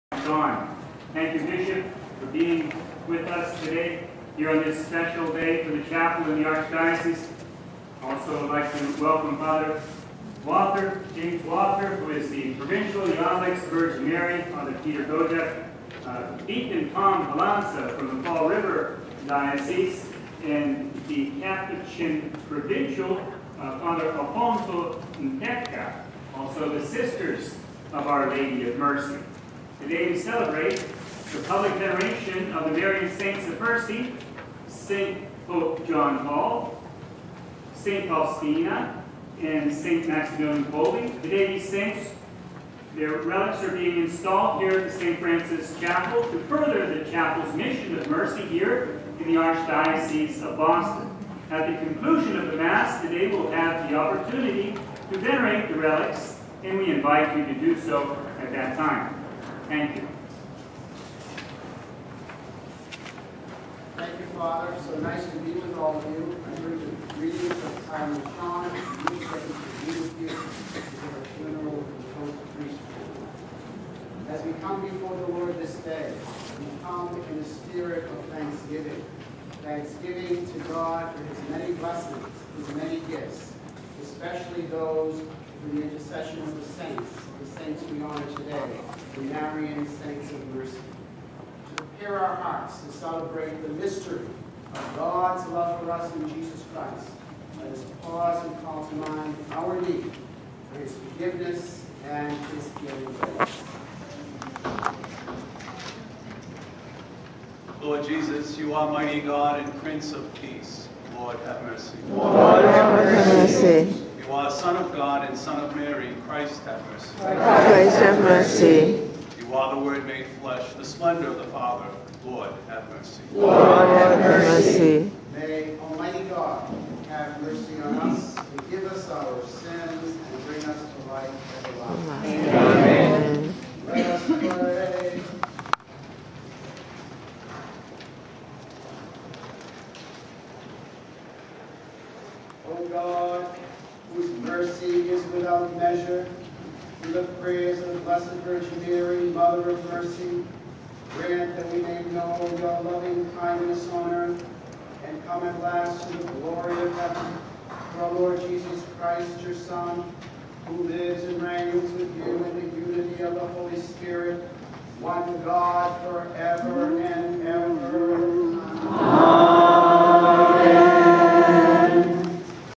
14 Introduction of Most Rev Peter Uglietto, celebrant by Very Rev. James Walther
Most Rev. Peter J  Uglietto is an Auxiliary Bishop of Archdiocese of Boston; Very Rev. James Walthers is Provincial of Oblates of the Virgin Mary (OMV).